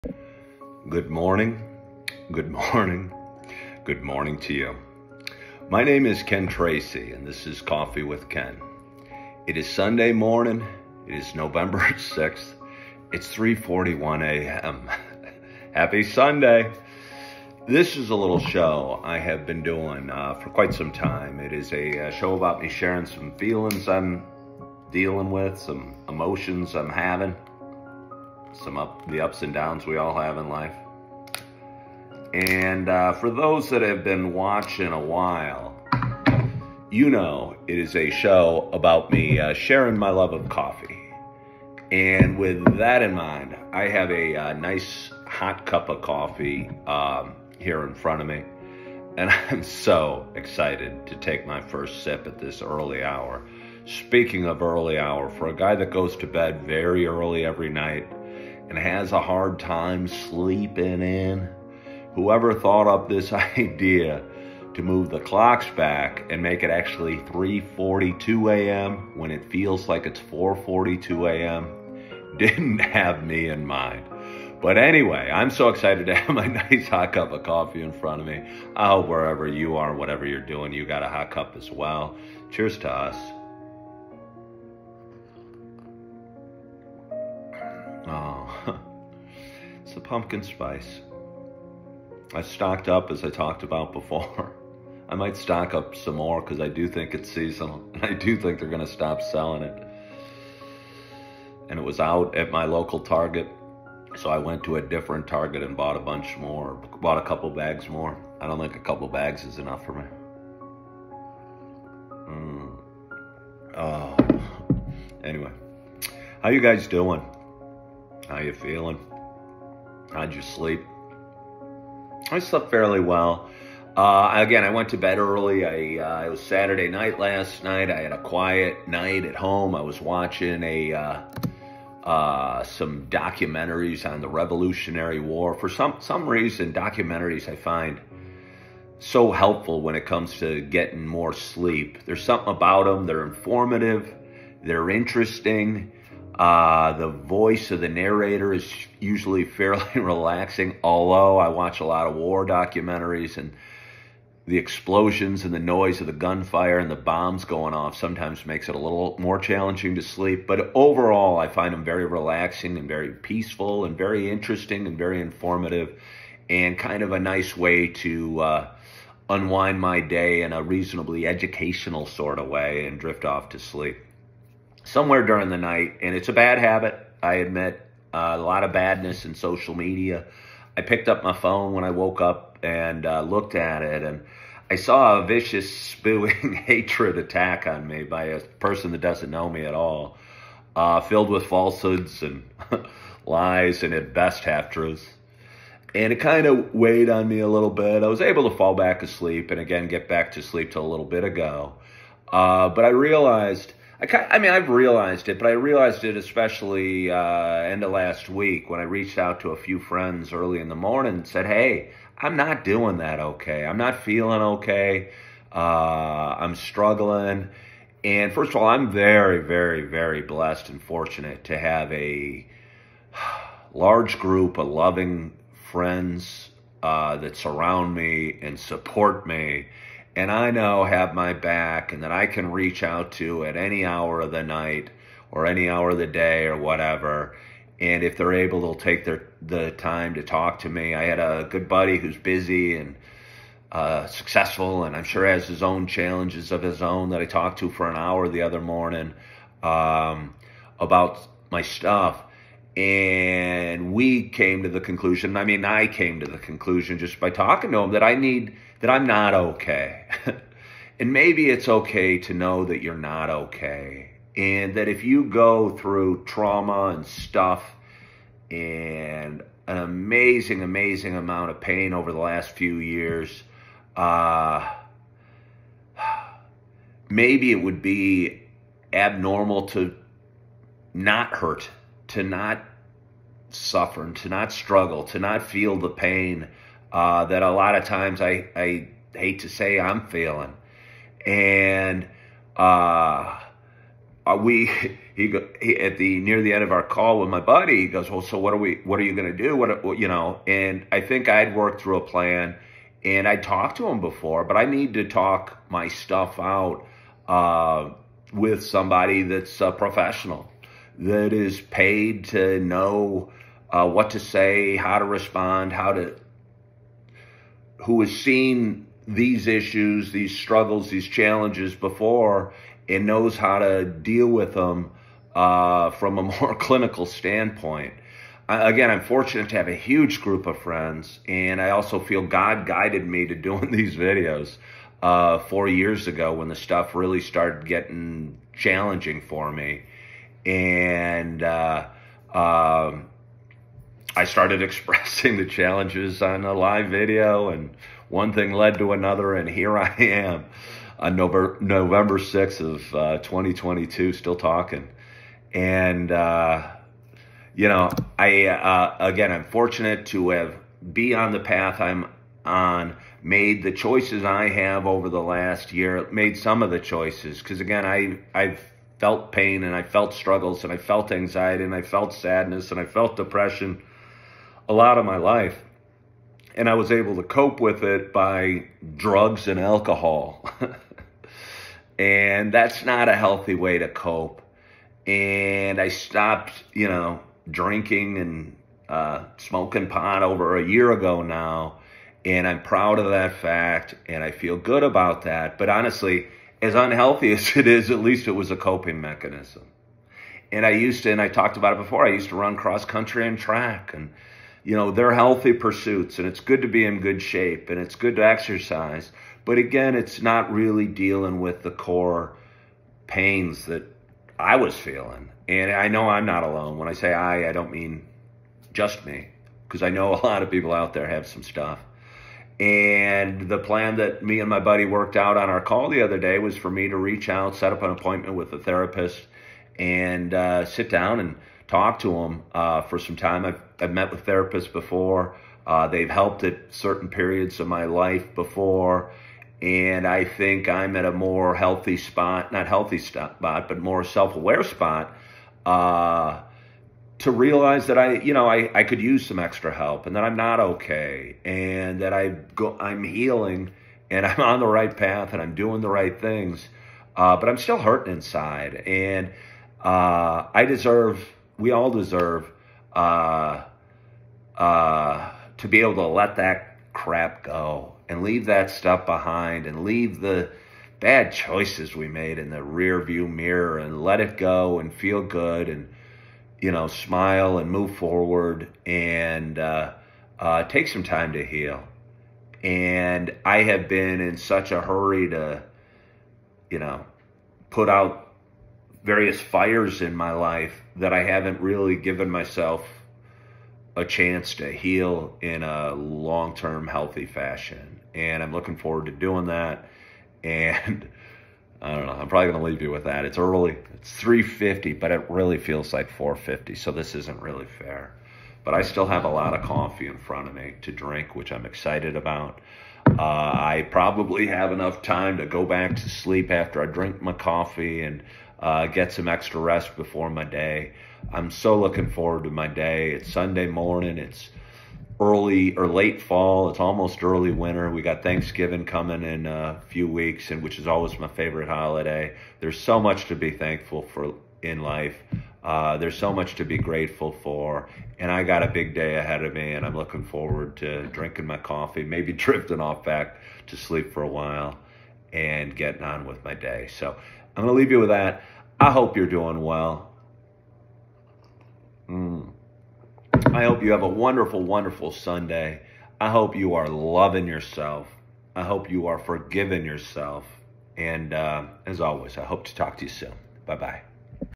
It is ok to know that you are not ok…
Join Ken Tracy as he discusses his own mental health and his desire to heal and speak with a therapist…